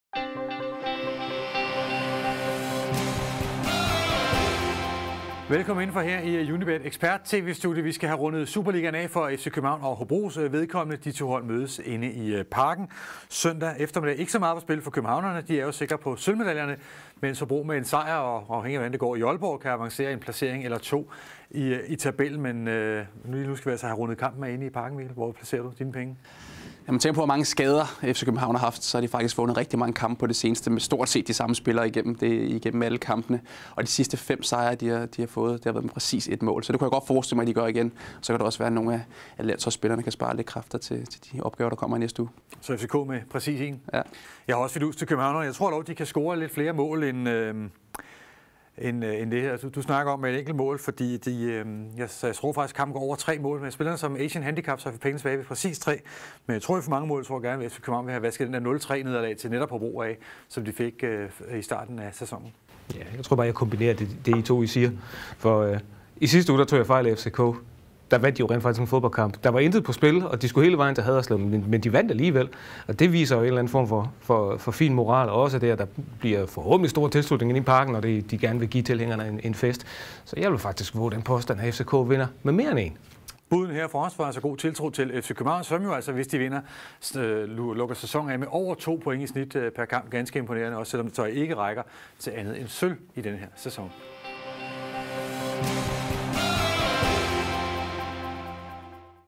Sikker sejr til FC København
Ekseprterne i Unibet Studiet er enige om, at københavnerne trækker det længste strå i kampen mod Hobro. De er dog ikke helt enige om sejrens størrelse. Se med her i klippet og få spiltips på kampen.